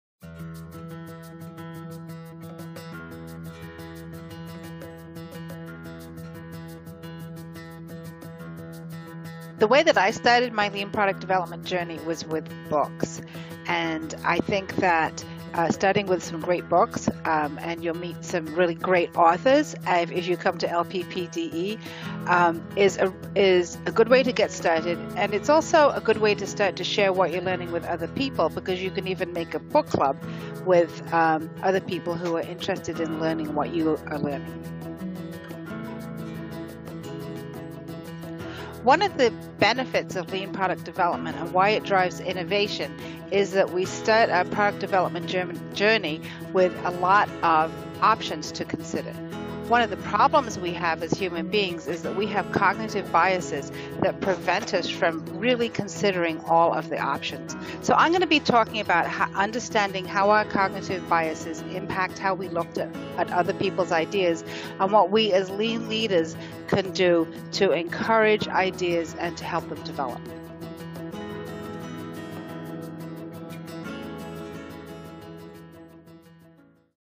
Bella Englebach Details Upcoming Lean Product Development Presentation
Join Bella in Columbus, OH October 1-4, 2018 at the Lean Product & Process Development Exchange!